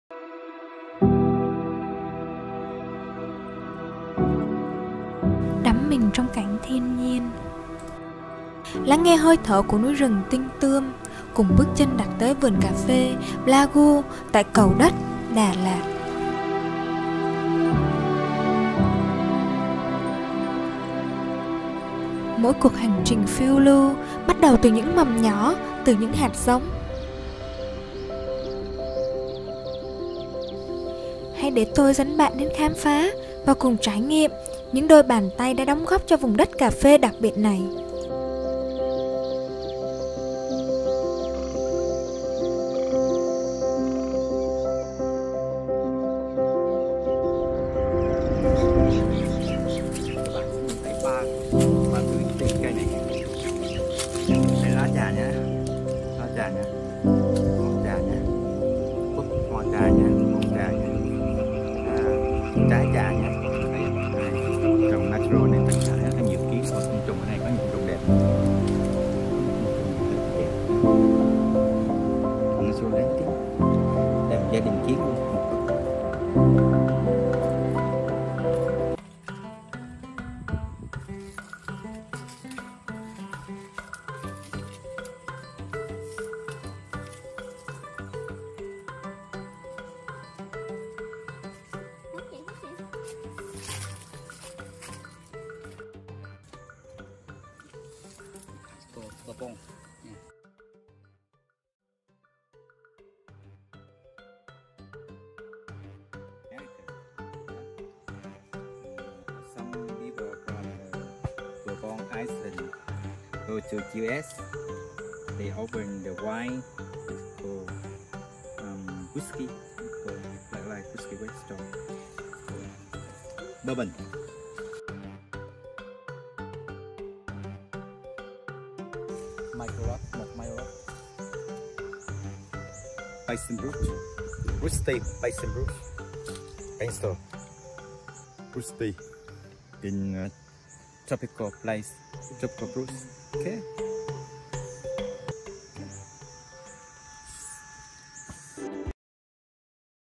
Farm To Cup - An unforgettable journey Part 1
Đặt chân lên vùng đất cao nguyên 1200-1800m so với mực nước biển khám phá nông trại cà phê tại vùng đất sương mù Đà Lạt.
Điều gì đã làm nên vùng đất cà phê này? Con người, khí hậu, …..
Ở tập này cùng theo chân Blagu khám phá vùng đất cà phê này và những giống cây tại nơi đây.